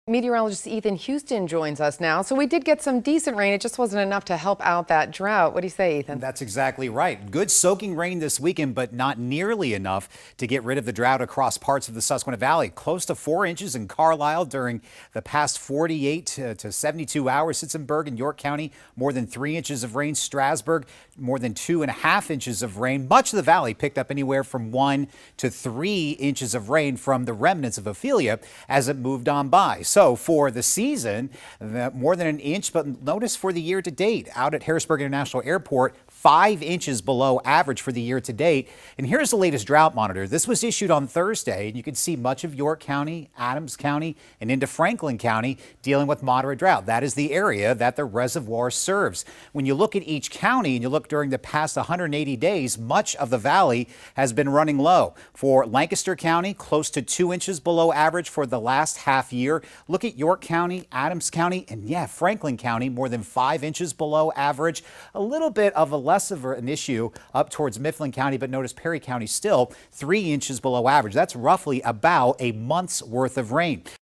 Weekend rainfall totals
News 8 meteorologist Ethan Huston has an update on rainfall in the Susquehanna Valley.